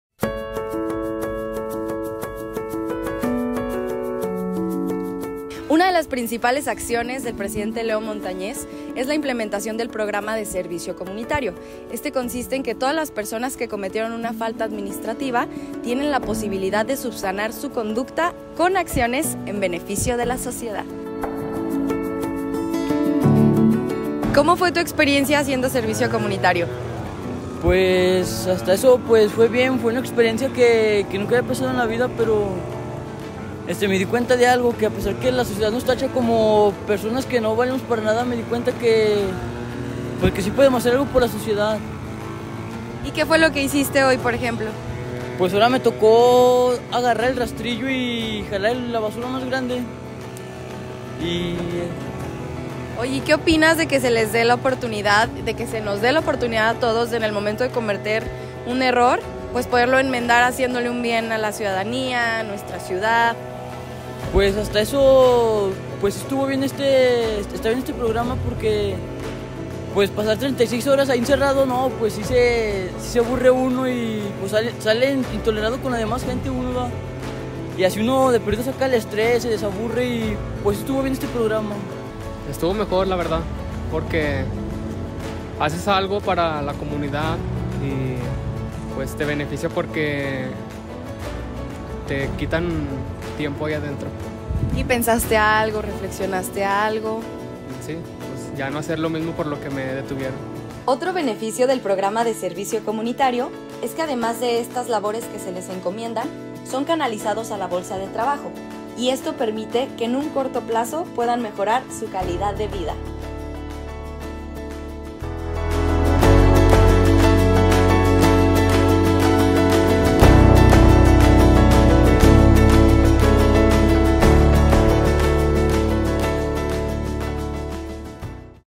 Leonardo Montañez promueve Servicio Comunitario a favor del orden, seguridad y limpieza
Aguascalientes, Ags. 7 de septiembre 2022.- Uno de los programas más significativos de la administración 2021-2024 que encabeza el presidente municipal Leonardo Montañez para que la ciudadanía cuente con espacios públicos más seguros, iluminados y ordenados para la convivencia y el tránsito peatonal, son los mega operativos de limpieza en el que se desarrolla el esquema de Servicio Comunitario, rehabilitándose en más de 60 jornadas más de un millón y medio de metros cuadrados, lo que equivale a 300 canchas de futbol soccer.
Como parte de la gira del Primer Informe de Gobierno del presidente municipal, Leonardo Montañez, se realizó un mega operativo de limpieza y servicio comunitario al sur de la ciudad en  avenida Héroe Inmortal, de donde se retiraron más de 30 toneladas de maleza y desechos en 5.46 kilómetros lineales, contando con la participación como en cada jornada de  personal de diferentes dependencias del Municipio, como Coordinación de Delegaciones, Obras Públicas, Medio Ambiente y Desarrollo Sustentable, Línea Verde, Protección Civil, Instituto Municipal Aguascalentense para la Cultura, y por supuesto la Secretaría de Servicios Públicos, así como vecinos voluntarios, organizaciones como scouts y la XIV Zona Militar, así como infractores del Servicio Comunitario.
De manera transversal la Secretaría de Servicios Públicos y la Dirección de Justicia Cívica llevan a cabo este esquema en el que han participado más de 5 mil 500 personas que cometen alguna infracción administrativa y tienen la oportunidad de cubrir su falta con actividades como limpieza de predios, desmalezado, barrido de calles y avenidas y mantenimiento general en diferentes zonas del municipio.
Cabe mencionar que en lo que comprende Tercer Anillo de Circunvalación, hasta Paseo Lomas del Sur, se dio mantenimiento a todo el alumbrado con alrededor de 1500 metros de cable para contar con iluminación más eficiente en este tramo.
Con estas acciones en que se conjuntan esfuerzos de la administración municipal y la sociedad se sensibiliza a las personas sobre la importancia del cuidado de los espacios públicos y el medio ambiente, para seguir siendo un municipio ejemplo por su limpieza, orden y participación social.
Al concluir el mega operativo en el que se contó con la presencia de la diputada Genny Janeth López Valenzuela, presidenta de la Comisión de Medio Ambiente, Recursos Naturales y Cambio Climático de la LXV Legislatura Local, Leonardo Montañez e integrantes del H. Cabildo y gabinete municipal, reafirmaron el compromiso de seguir  trabajando intensamente para que cada vez más personas cuiden y trabajen por un mejor Aguascalientes.